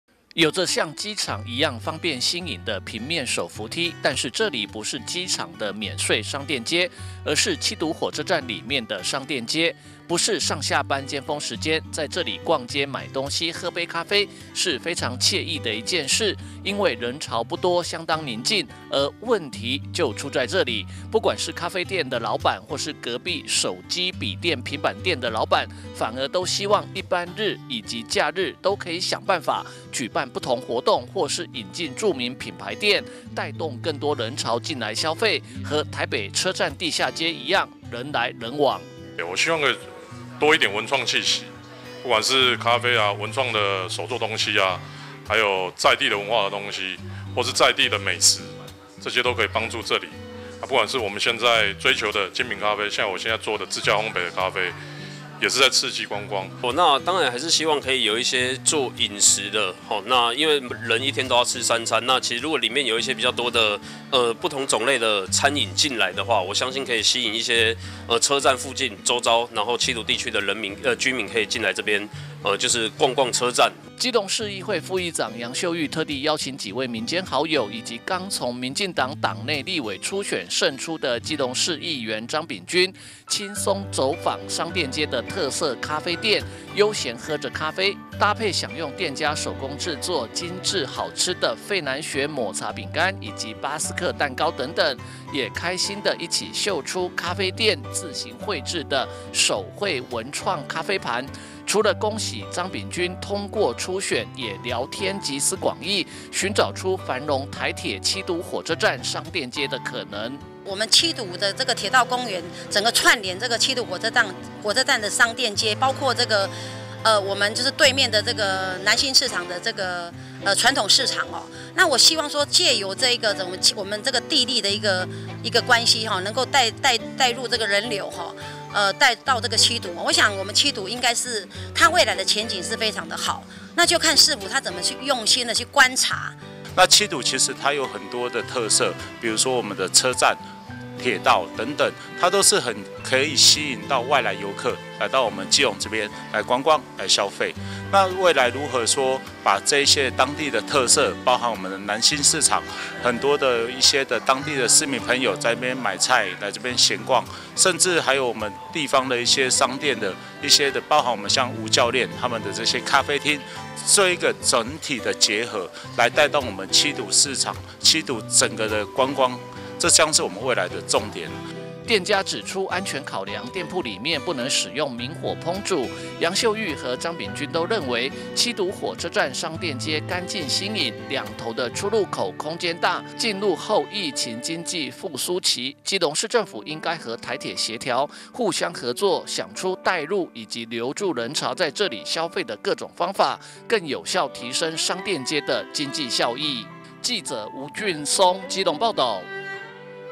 基隆|楊秀玉|張秉鈞|七堵火車站|商店街|咖啡|手機|平板|楊秀玉邀張秉鈞喝咖啡 訪七堵火車站商店街
楊秀玉邀張秉鈞喝咖啡 訪七堵火車站商店街

(中嘉吉隆新聞記者吳俊松/基隆報導)如何帶動七堵火車站商店街的人潮，連帶結合七堵區中心的觀光消費，一直是基隆市議會副議長楊秀玉積極推動的事，因此特地邀請幾位民間好友以及剛從民進黨黨內立委初選勝出的基隆市議員張秉鈞，輕鬆走訪商店街的特色咖啡店喝咖啡，除了恭賀秉鈞通過初選也聊天集思廣益，尋找出繁榮台鐵七堵火車站商店街的可能。

有著像機場一樣方便新穎的平面手扶梯，但是這裡不是機場的免稅商店街，而是七堵火車站裡面的商店街，不是上下班尖峰時間，在這裡逛街買東西喝杯咖啡是非常愜意的一件事，因為人潮不多相當寧靜，而問題就出在這裡，不管是咖啡店的老闆或是隔壁手機筆電平板店的老闆反而都希望一班日及假日都可以想辦法舉辦不同活動或是引進著名品牌店帶動更多人潮進來消費，和台北車站地下街一樣人來人往！

基隆市議會副議長楊秀玉特地邀請幾位民間好友以及剛從民進黨黨內立委初選勝出的基隆市議員張秉鈞，輕鬆走訪商店街的特色咖啡店悠閒喝著咖啡，搭配享用店家手工製作精緻好吃的費南雪抹茶餅乾以及巴斯克蛋糕，也開心的一起秀出咖啡店自行繪製的手繪文創咖啡盤！除了恭賀秉鈞通過初選也聊天集思廣益，尋找出繁榮台鐵七堵火車站商店街的可能。

店家指出，安全考量，店鋪裡面不能使用明火烹煮，楊秀玉和張秉鈞都認為七堵火車站商店街乾淨新穎，兩頭的出入口空間大，進入後疫情經濟復甦期，基隆市政府應該和台鐵協調，互相合作，想出帶入以及留住人潮在這裡消費的各